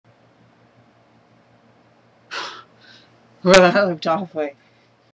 Lucy Side 5.wmv
A reaction audition to a staged decapitation that was a little too convincing.